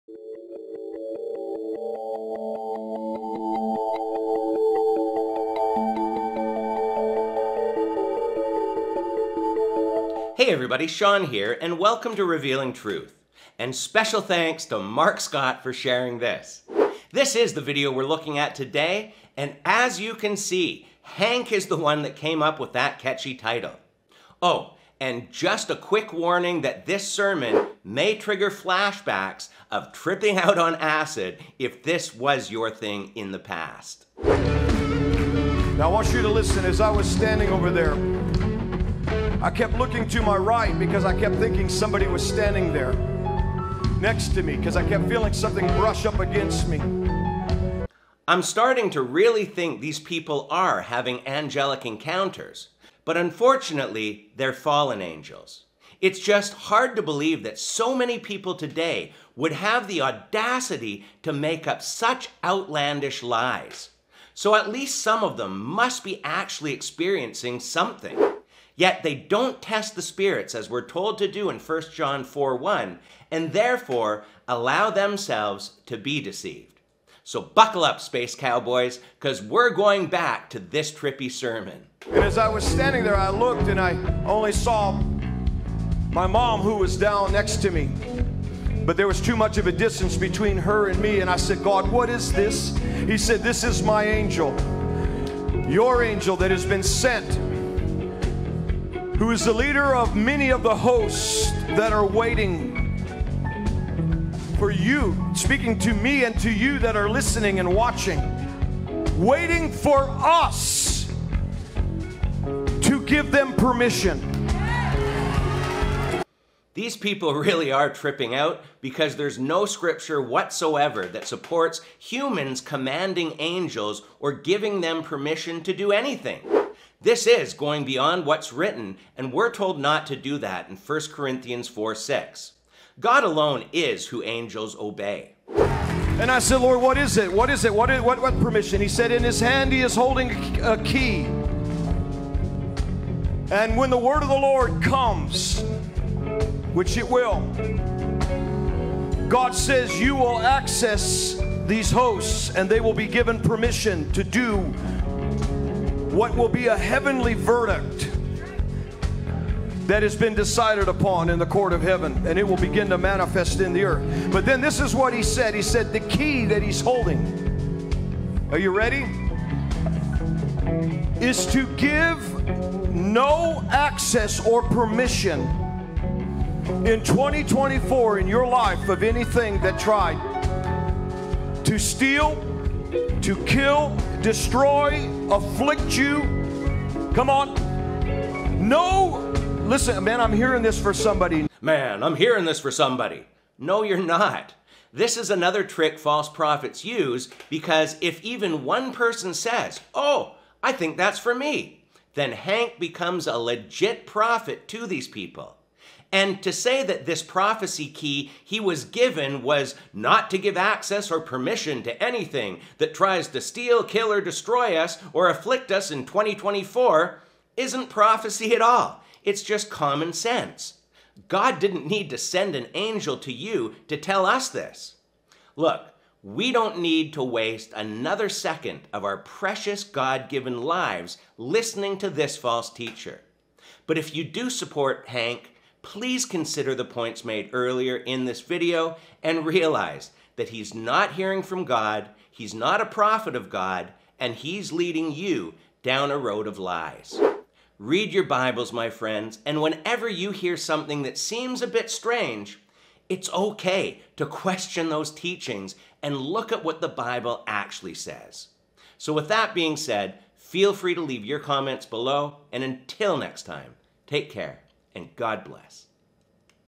An Angel Stood Beside Hank With a 2024 Prophecy
False Trump Prophet Hank Kunneman continues to lead the flock astray with bogus prophecy in 2024.